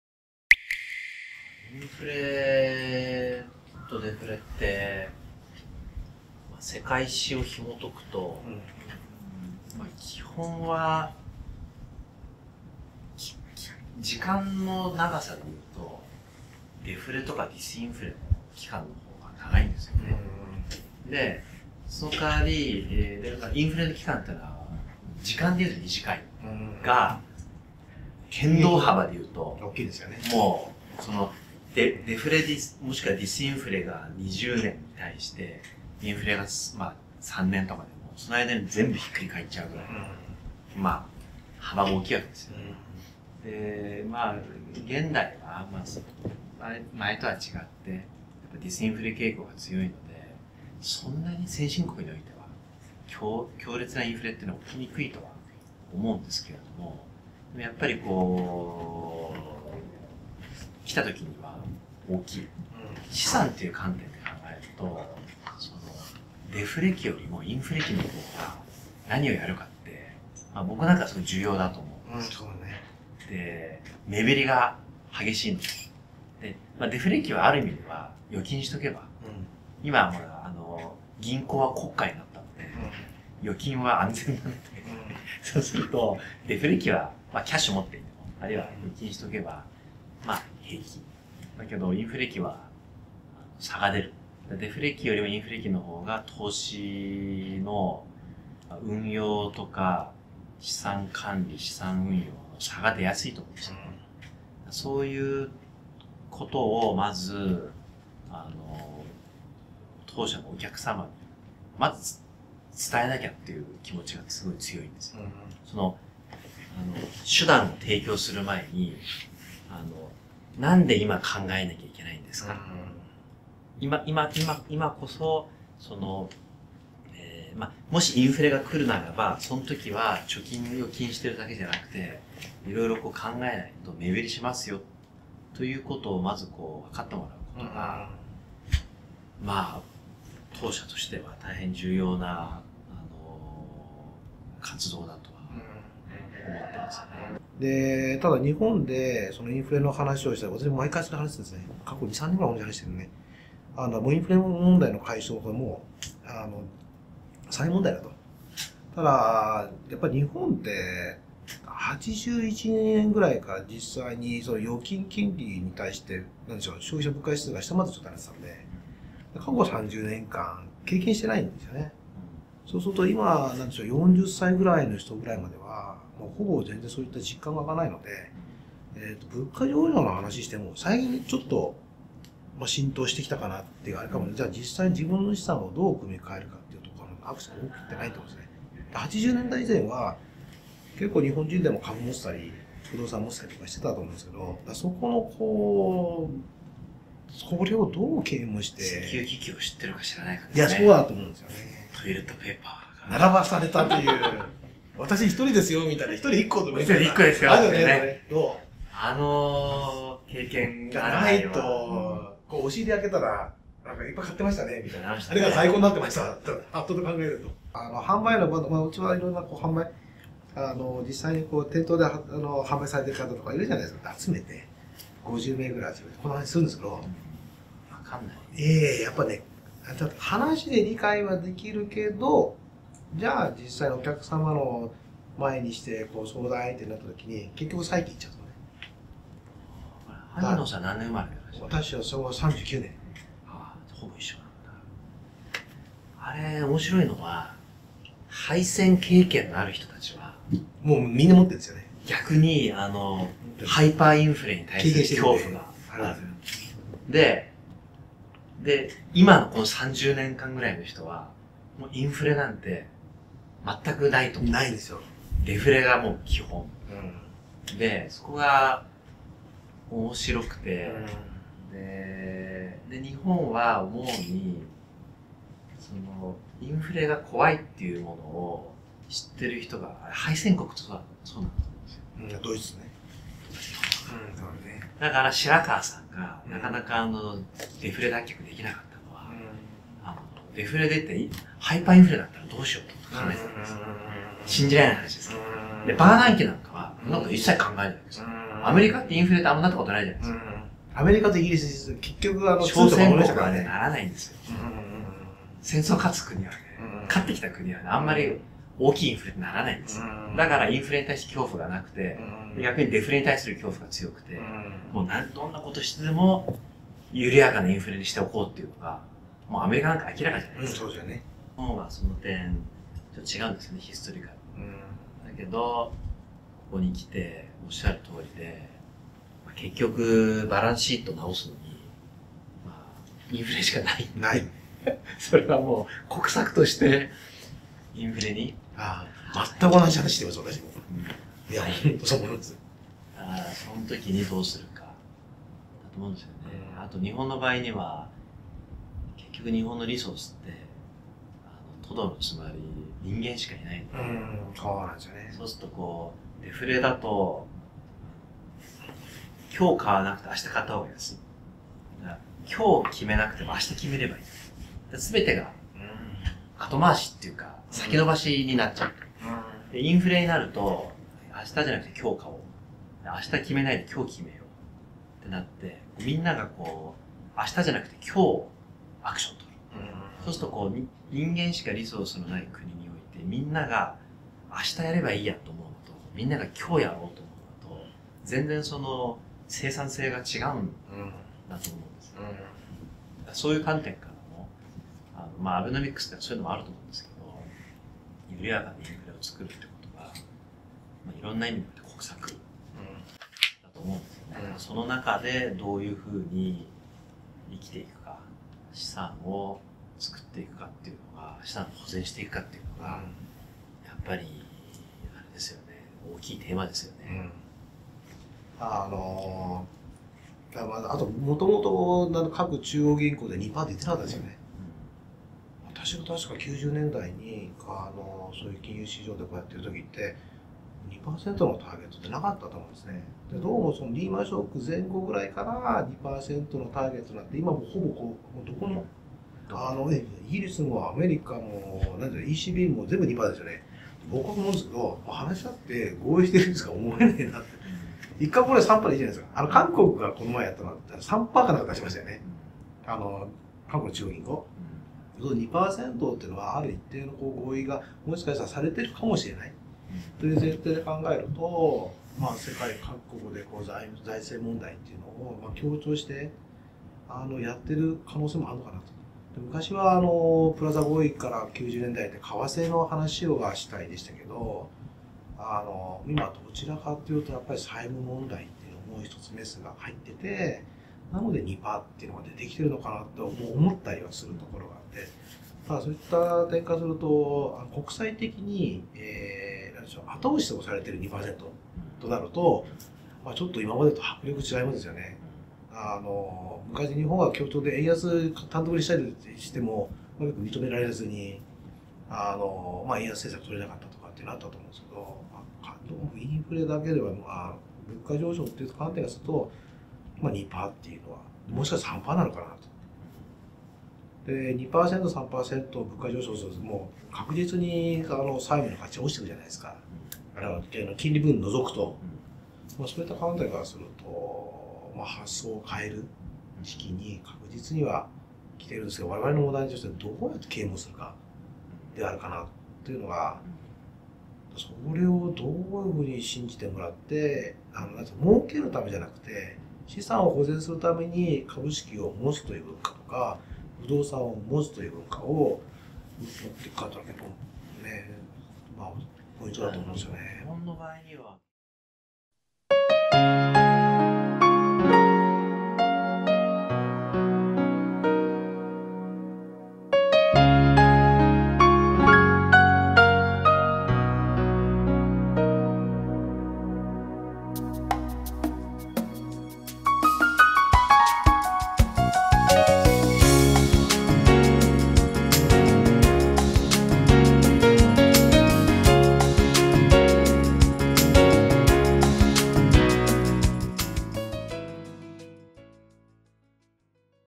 2014/6 Special Interview #01 松本大 x 萩野琢英
マネックス証券の松本大社長とピクテ投信投資顧問の萩野琢英のスペシャルインタビュー！2人が語る日本のインフレについて・・・。そしてお2人から皆様へのメッセージも！